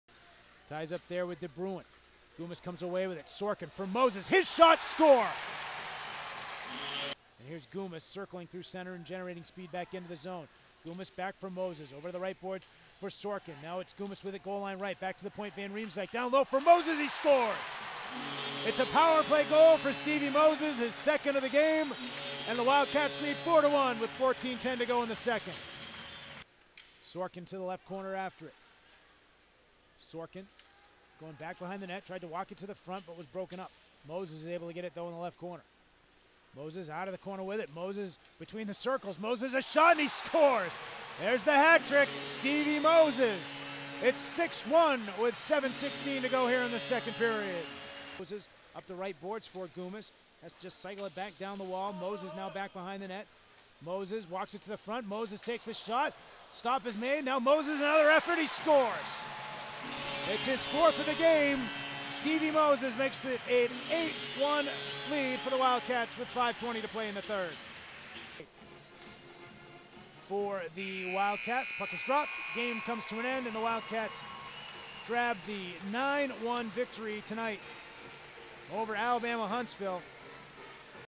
MOSES SCORES FOUR GOALS AS NEW HAMPSHIRE TOPS ALABAMA HUNTSVILLE, 9-1
Senior forward Stevie Moses scored four goals and five other Wildcats posted multiple-point games as New Hampshire skated to a 9-1 win against Alabama-Huntsville on Saturday, November 26, 2011 at the Whittemore Center.